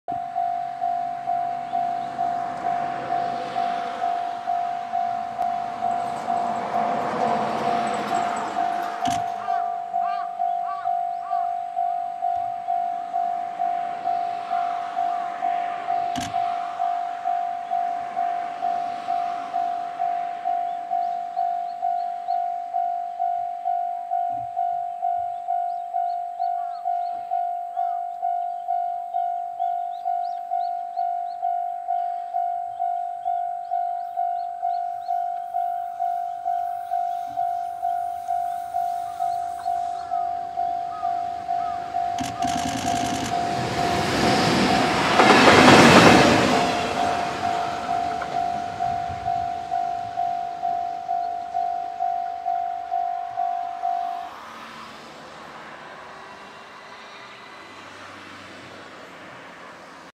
青い森鉄道 EH500形+チキ6000形 3079レ 苫米地～北高岩 2022年7月25日
牽引機 EH500-11
チキ2車 八戸貨物行き保全レール輸送